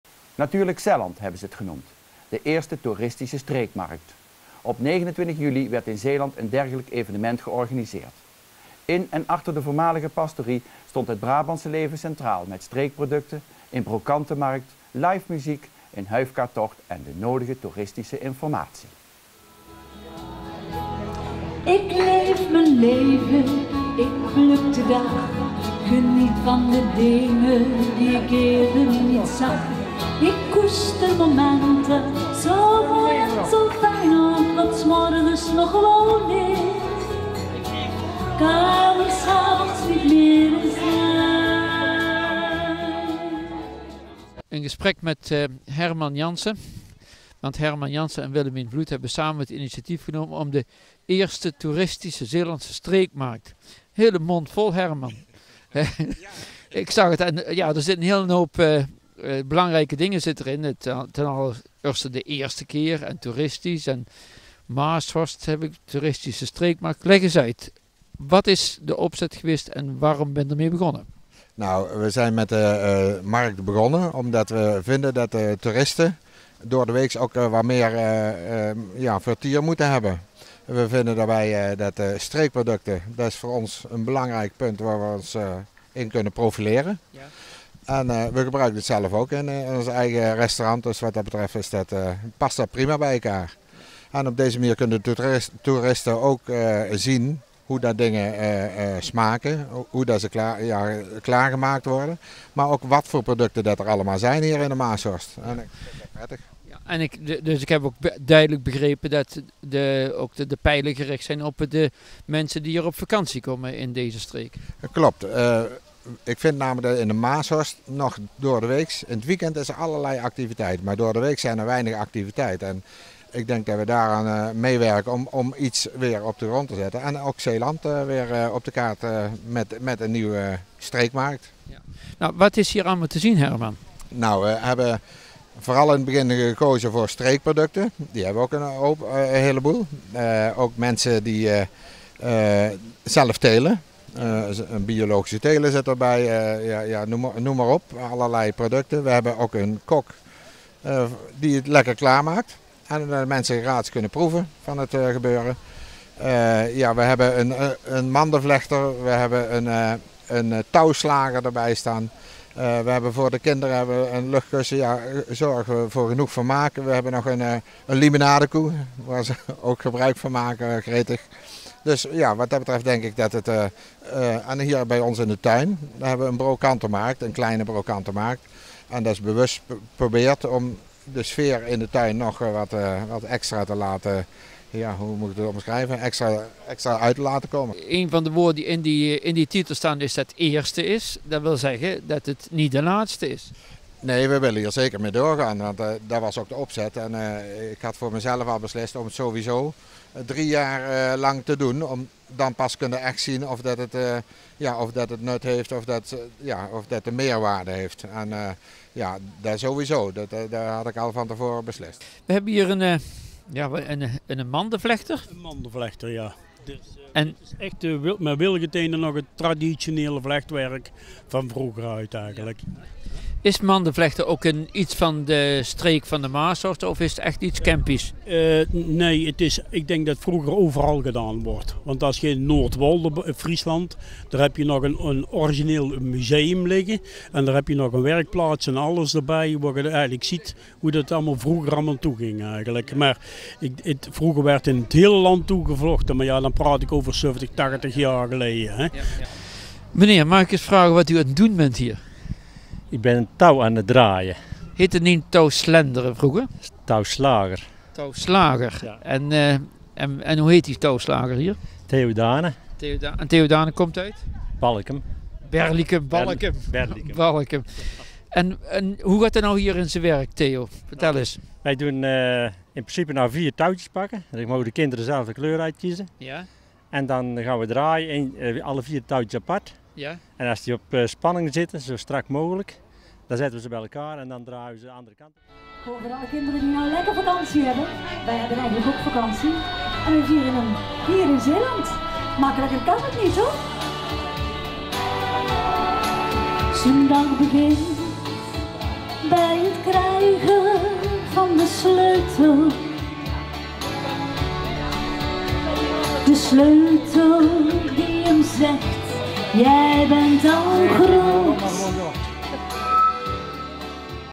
Natuurlijk Zelland
Natuurlijk Zelland, hebben ze het genoemd. De eerste toeristische streekmarkt. Op 29 juli werd in Zeeland een dergelijk evenement georganiseerd. In en achter de voormalige pastorie  stond het Brabantse leven centraal met streekproducten, een brocante markt, live muziek, een huifkartocht en  de nodige toeristische informatie.